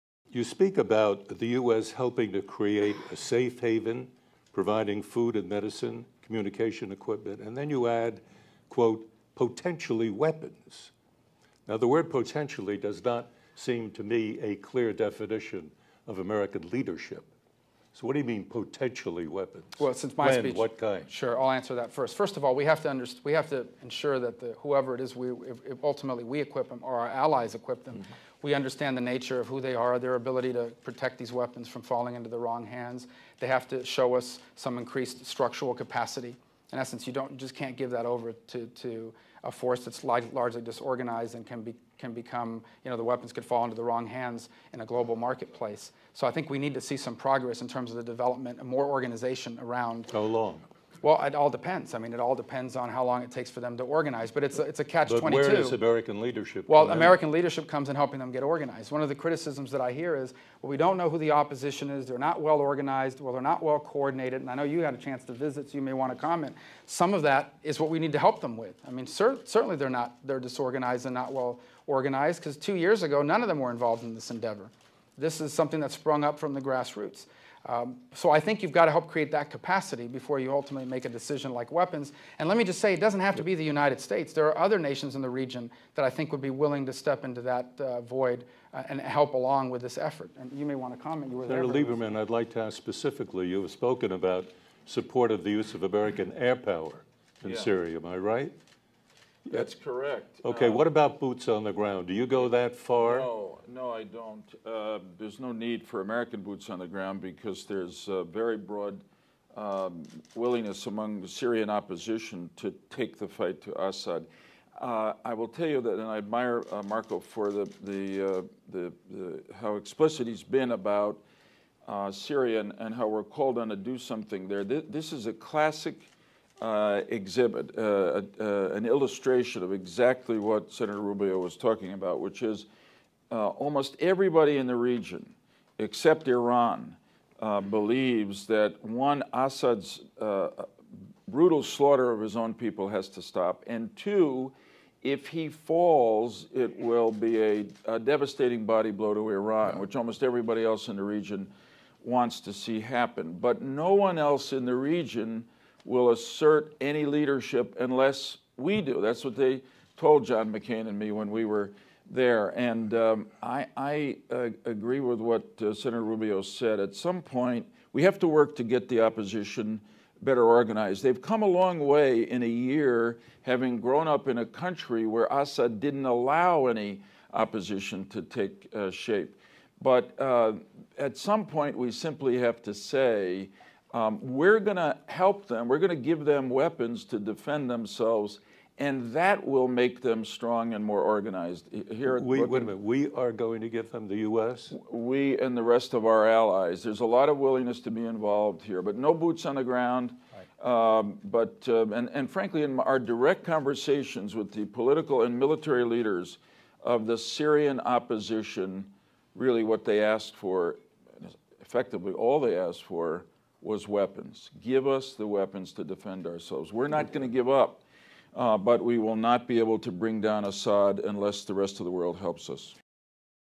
An Address by Senator Marco Rubio: Ending the Crisis in Syria
On April 25, Foreign Policy at Brookings hosted Senator Marco Rubio (R-Fla.) for a major address on the future of U.S. foreign policy.

In this Q&A segment, Senator Rubio says Syria is in dire straits and American leadership is critical to helping bring an end to the situation there.. For more on this event including full video, audio, and transcript